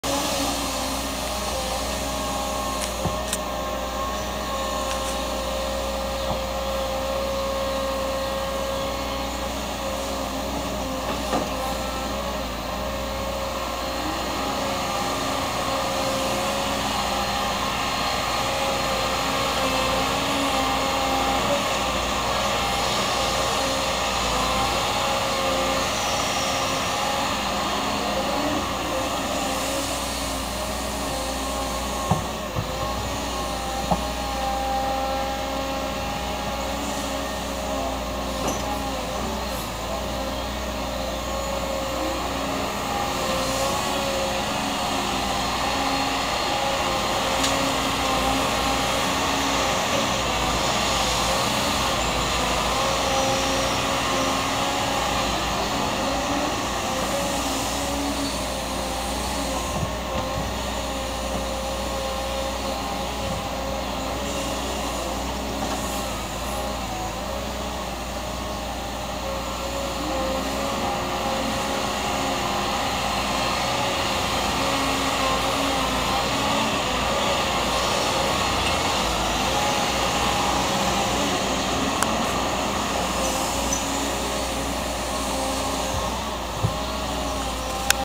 (MFØ) | Fjernvarmegraving ved Folkenborg ikke langt fra Mysen
Prosjekt: Fjernvarme graving for Kommunen
Bedrift/Firma: Tune Graveservice AS
Dato: 26.05.
År: 2014
Filmet av: Maskinfotografen Østfold
Del 2